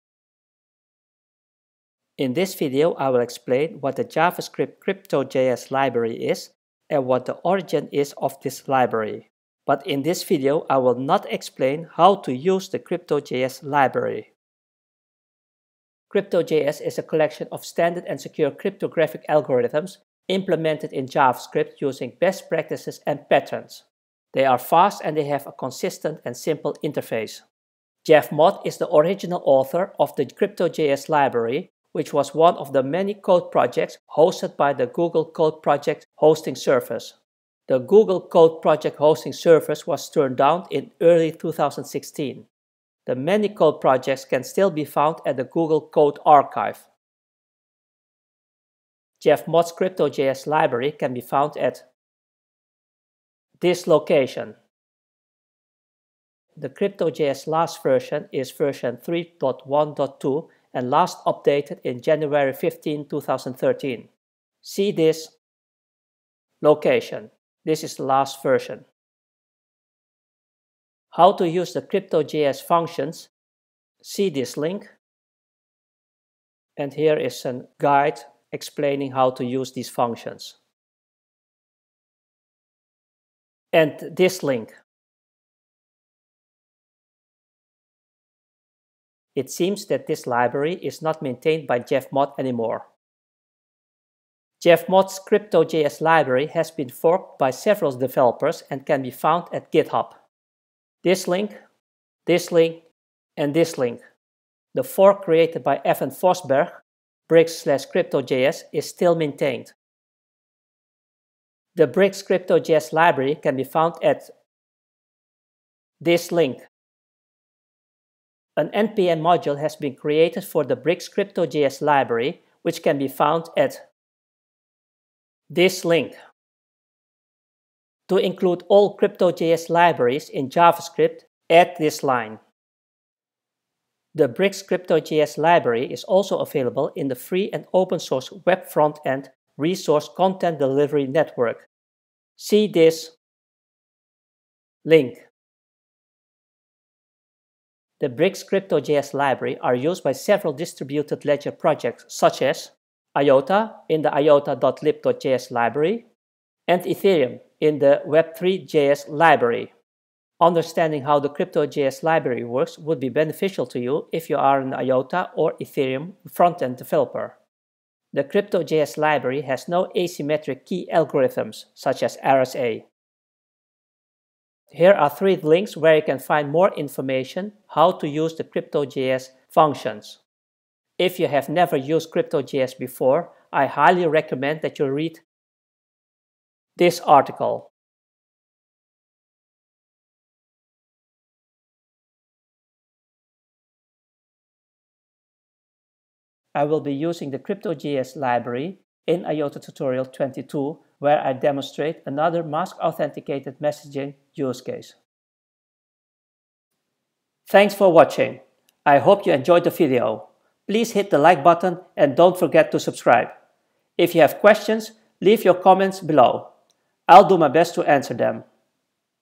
IOTA tutorial 21: CryptoJS
If you like this video and want to support me, go this page for my donation crypto addresses:

This is part 21 of the IOTA tutorial.

In this video series different topics will be explained which will help you to understand IOTA.
It is recommended to watch each video sequentially as I may refer to certain IOTA topics explained earlier.

The main objective of this video is to provide you with some basic knowledge about CryptoJS.

CryptoJS is a collection of standard and secure cryptographic algorithms implemented in JavaScript using best practices and patterns.
They are fast and they have a consistent and simple interface.
Jeff Mott is the original author of the crypto-js library which was one of the many code projects hosted by the Google Code Project Hosting Service.
The Google Code Project Hosting Service was turned down in early 2016.
The many code projects can still be found at the Google Code Archive.

The CryptoJS last version is v3.1.2 and last updated in Jan 15, 2013.

It seems that this library is not maintained by Jeff Mott anymore.

Jeff Mott’s CryptoJS library has been forked by several developers and can be found at Github:
The fork created by Evan Vosberg (brix/crypto-js) is still maintained.

An npm module has been created for the (Brix) CryptoJS library which can be found at:

To include all CryptoJS libraries in JavaScript:
var CryptoJS = require(“crypto-js”);

The (Brix) CryptoJS library is also available in the free and open source web front-end resource Content Delivery Network (CDN):

The (Brix) CryptoJS library are used by several Distributed Ledger projects, such as:
- IOTA (in the iota.lib.js library)

Understanding how the CryptoJS library works would be beneficial to you if you are an IOTA or Ethereum front-end developer.
The CryptoJS library has no asymmetric key algorithms, such as RSA.